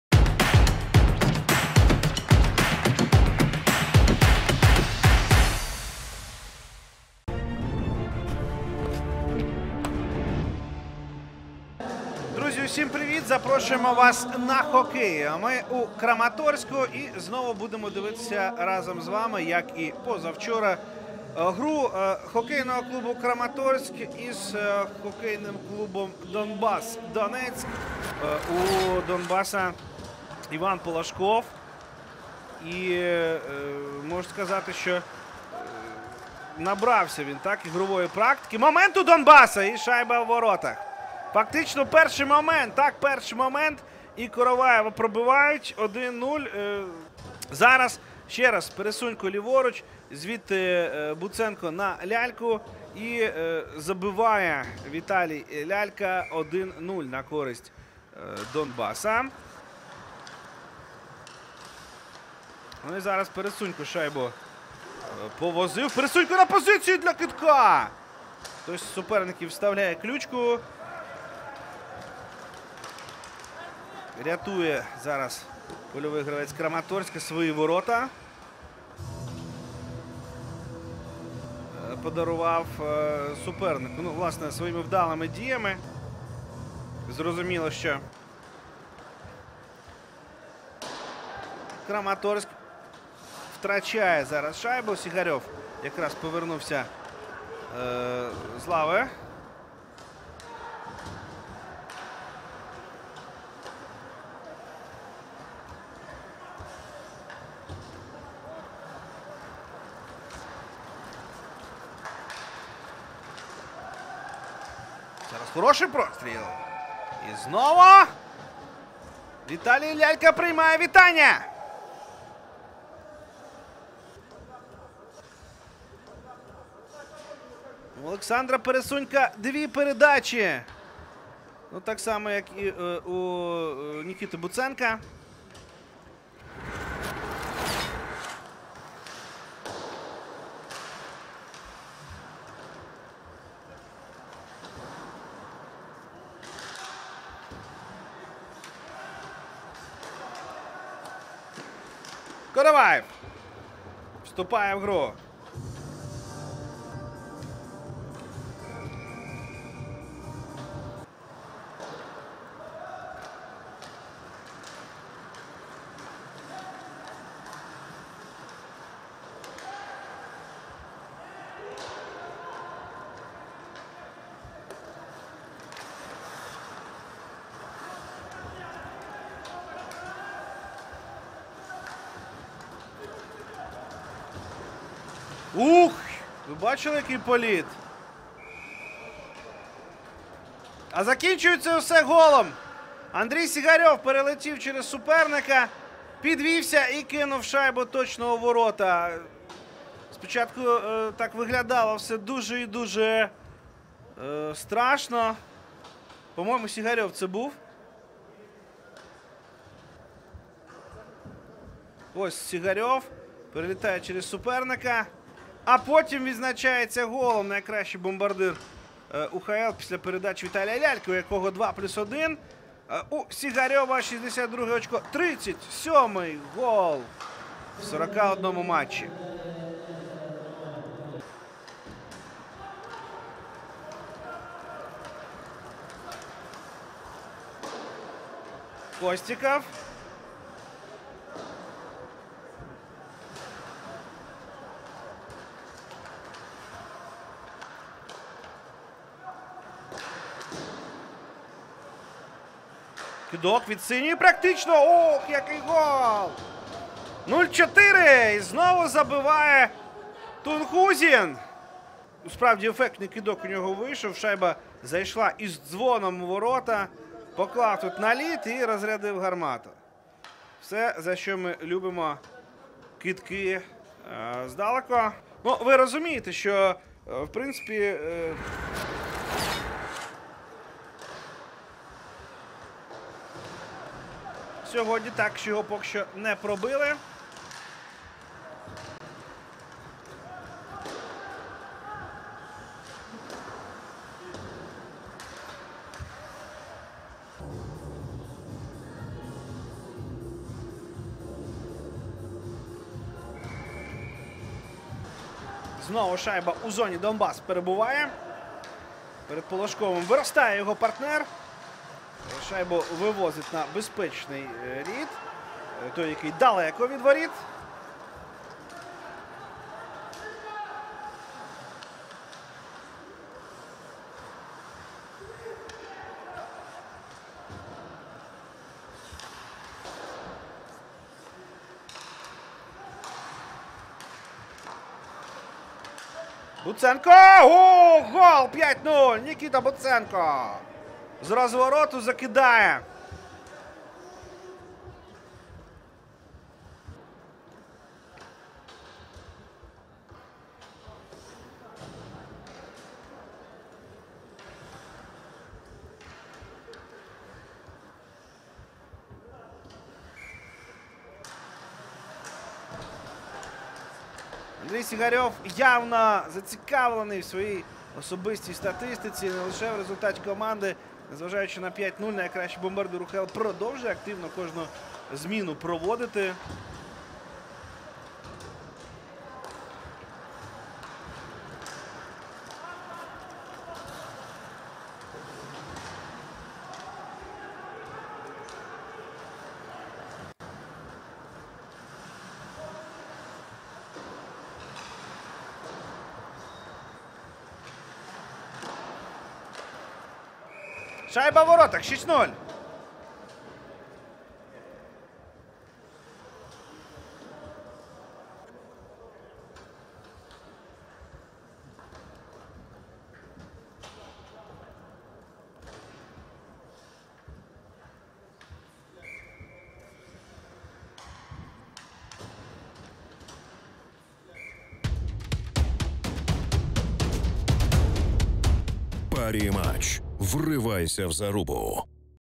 15 тур УХЛ Париматч Краматорск - Донбасс 0:6
Лучшие моменты матча 15-го тура Украинской хоккейной лиги Париматч 2020/21 «Краматорск» - «Донбасс».